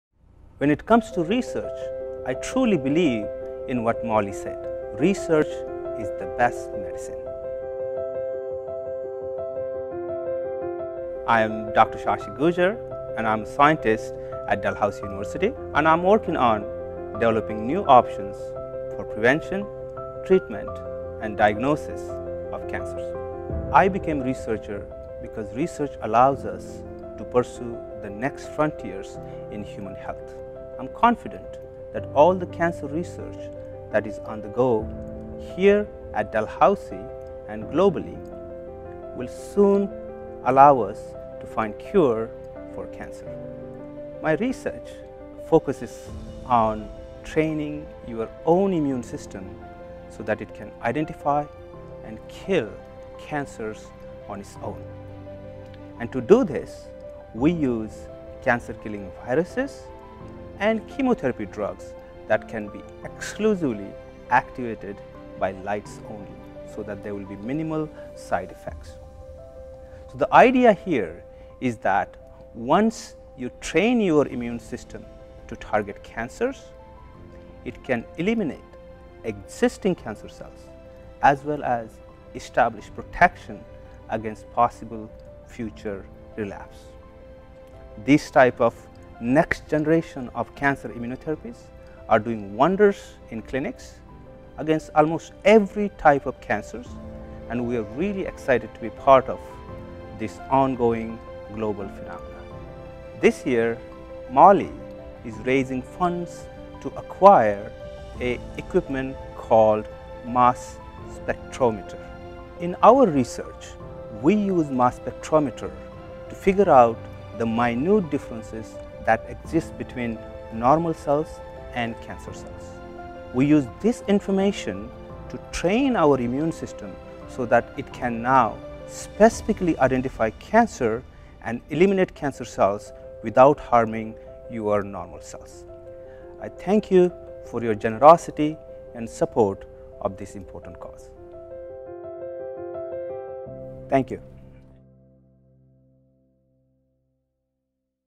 DMRF Molly Appeal Researcher: Dr. Shashi Gujar
“Our goal is to develop personalized therapies that educate
patients’ immune systems to recognize and destroy cancer.
We are exploring how to use cancer-killing viruses – which
target and kill cancer cells without harming healthy cells --
to flag cancer cells to the immune system. When we infect
cancer cells with the virus, it’s like putting a transponder
in the cancer cell. Now the immune system recognizes the
signature of that cancer and will target and kill not just that
cell but all other cancer cells like it.”